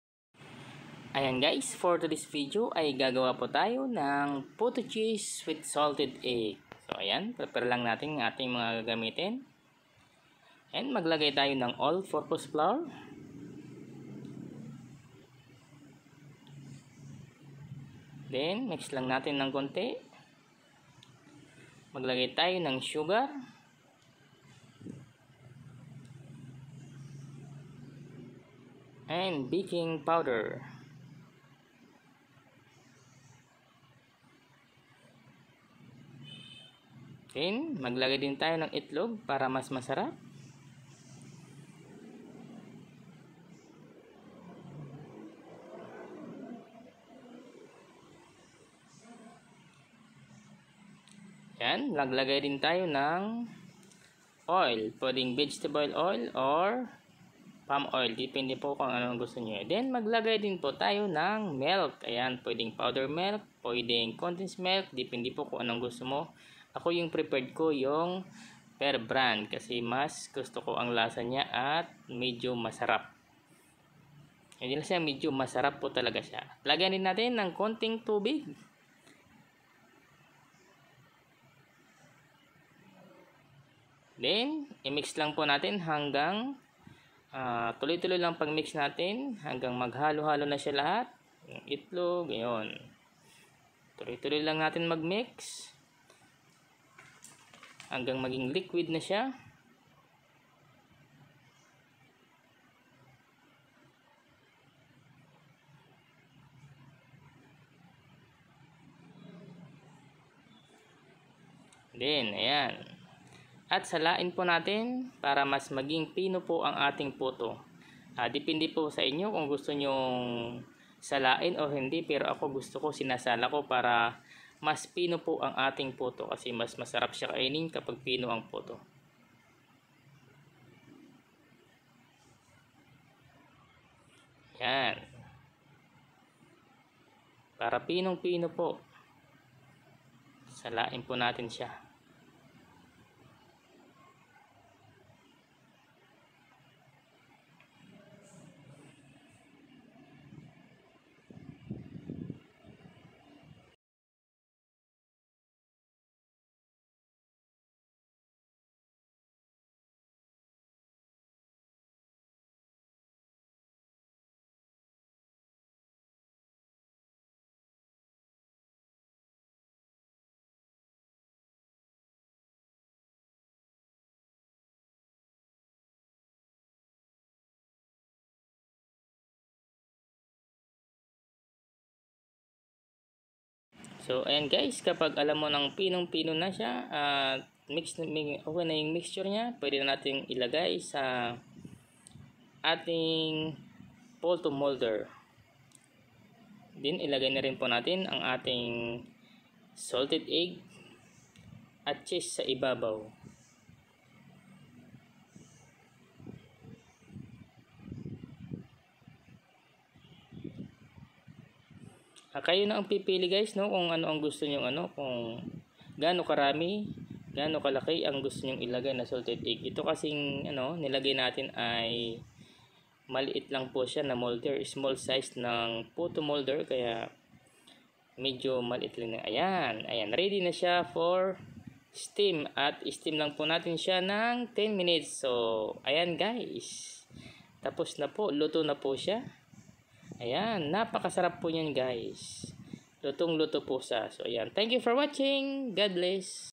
Gawa Tayo ng Puto Cheese with Salted Egg
Try nyo gumawa guys ang sarap nito.